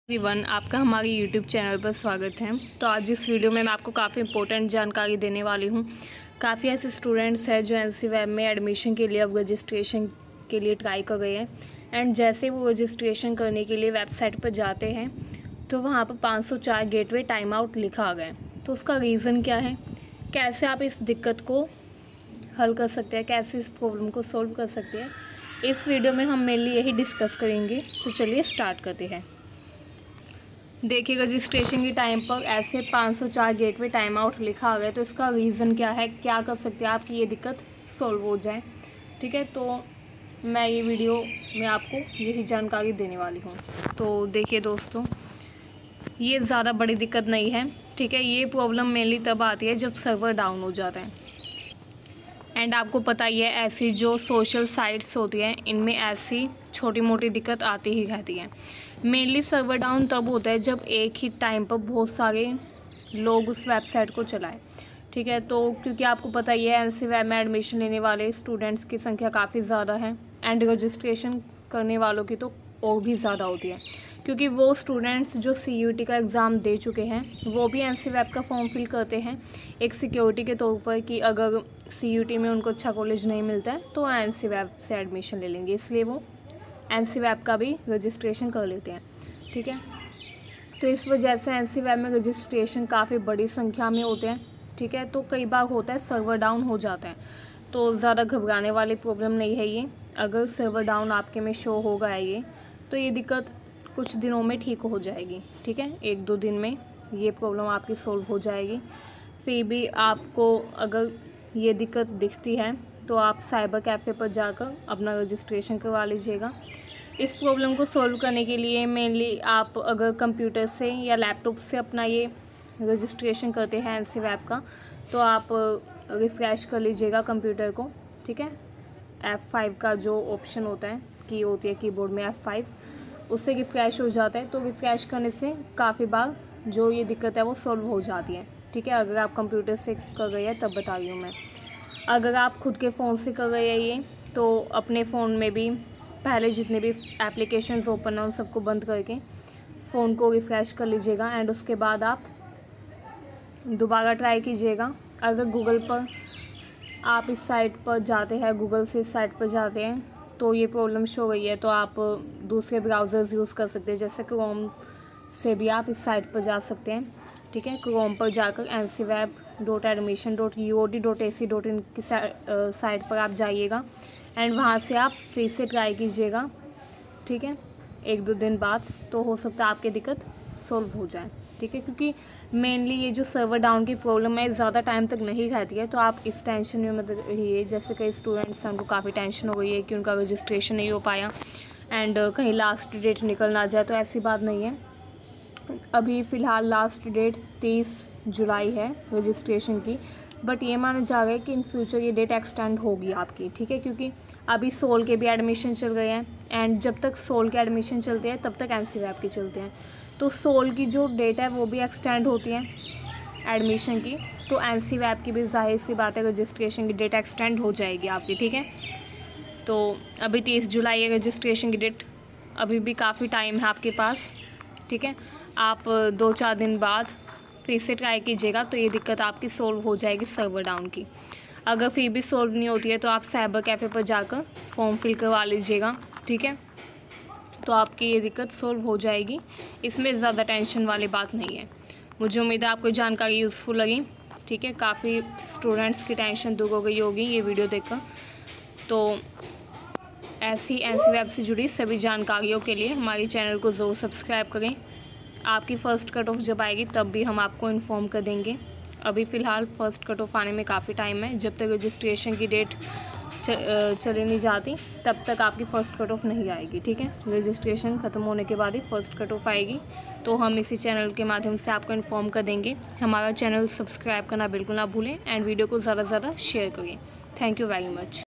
Ncweb Admission.. 504 Gateway timeout show hone ka karan.. how to fix this problem.
hello students

In this video I will tell you about reason of 504 gateway time out notification being shown on ncweb site during registration..

I will also tell you about how to fix this problem..

ncweb admission
ncweb admission 2023
problem of server down
how to fix this problem

hopefully you liked this video